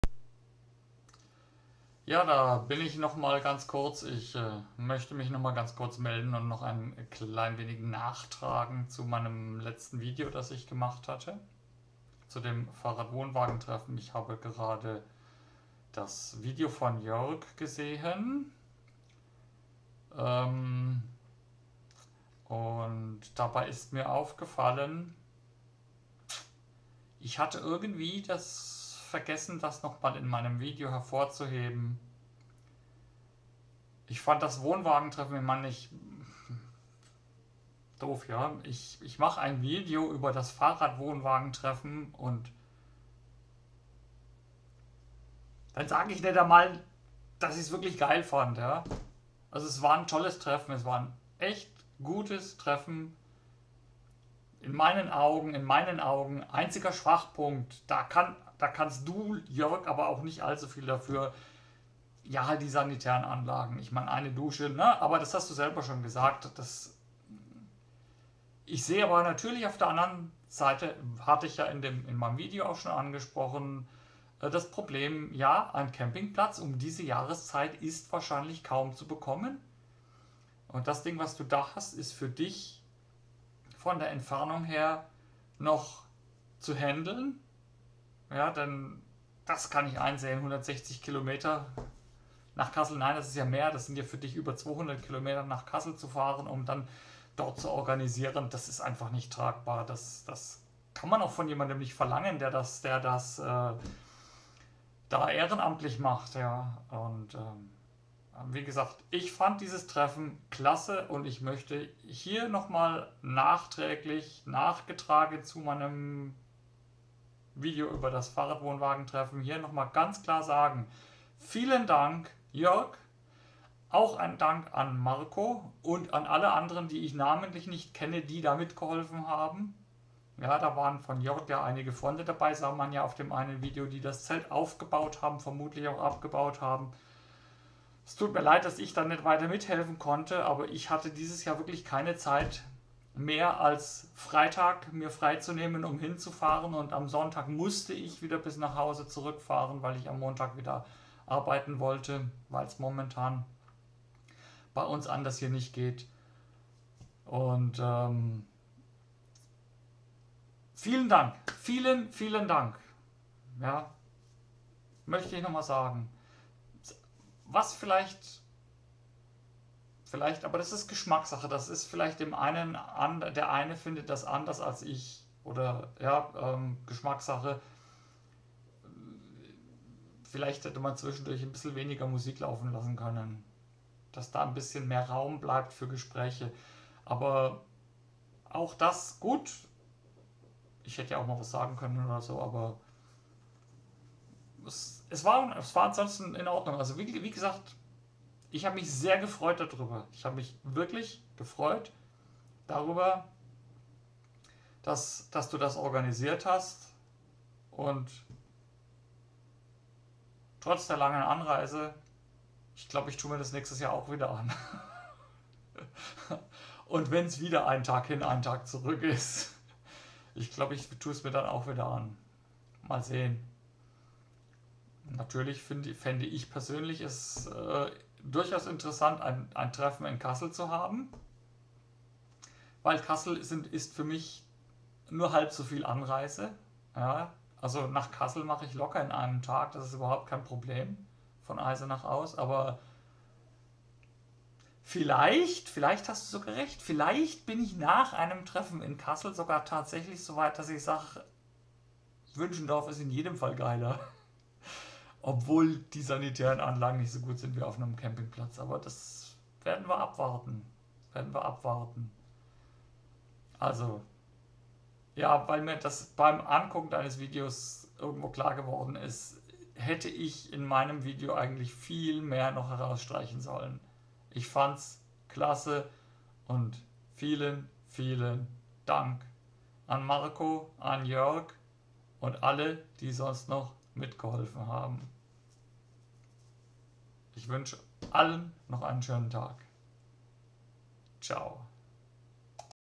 Short - Nachtrag zum Fahrradwohnwagentreffen in Wünschendorf
Ein paar Gedanken, mir nach dem Ansehen von Jörgs Video "Reaktion auf AlexTek - FaWoWa Treffen 2023 in Kassel" auf dem Herzen brennen.